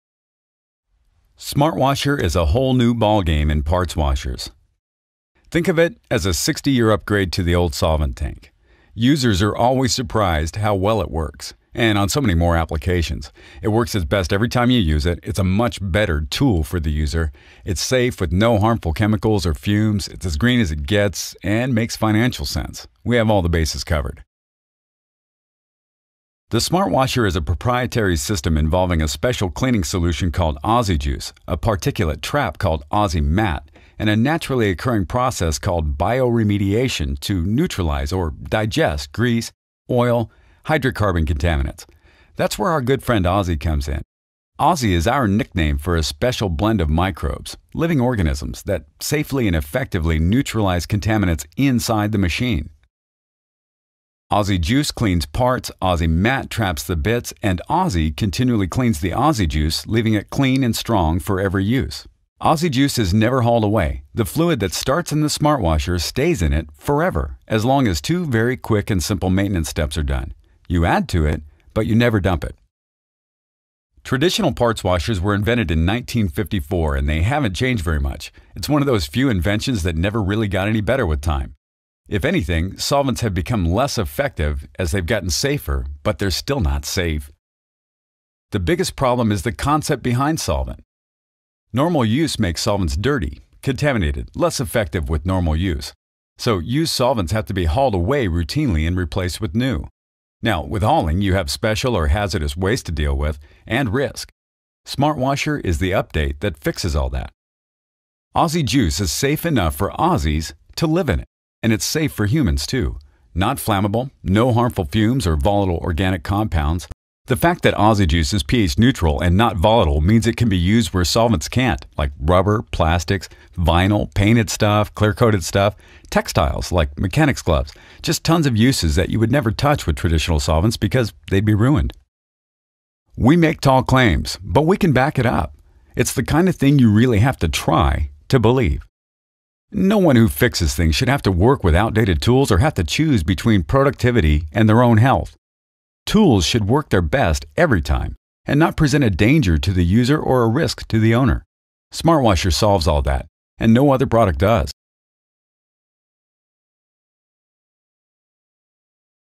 Smartwasher® - The Sustainable Cleaning Solution
The SmartWasher Bioremediating Parts Washing System is both self-cleaning and environmentally friendly.
The pH neutral cleaner called OzzyJuice is used to effectively clean parts, removing grease and grime while the naturally occurring process of bioremediation is used to turn those potentially hazardous contaminants into a harmless by-product of carbon dioxide and water.
Simply put, the OzzyJuice cleans the part while the bioremediation process cleans the fluid.
Check out our video to get the full run-down on why this is the best sustainable cleaning solution for you.